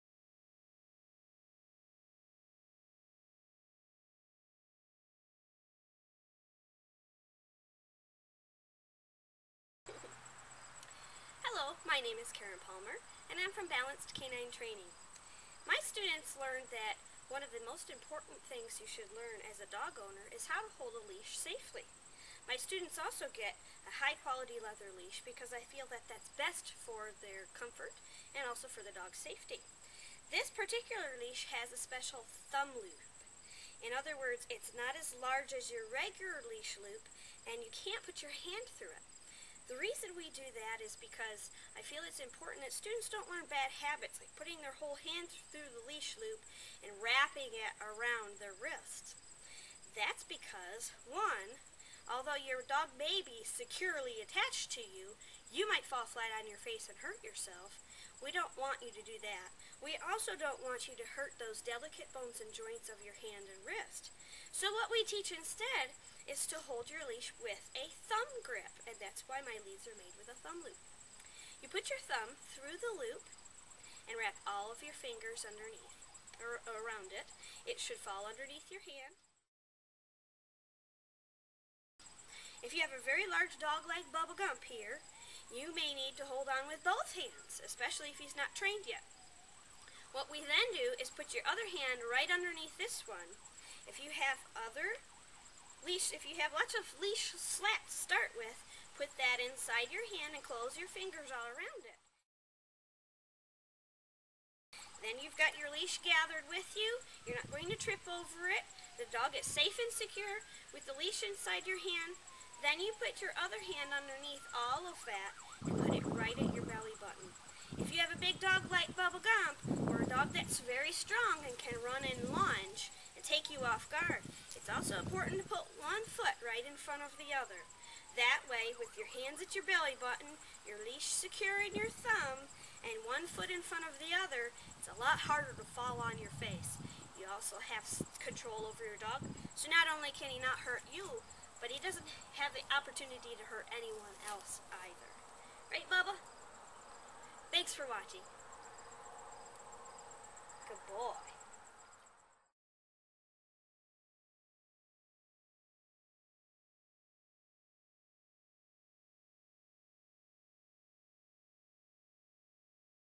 Leash Safety.wmv
This video is connected to a free handout on our website's "Articles" page. These tips are most important for owners and handlers of large, untrained dogs, but helpful for the comfort and safety of all handlers and dogs. for more information on the fit and use of various dog training and management tools.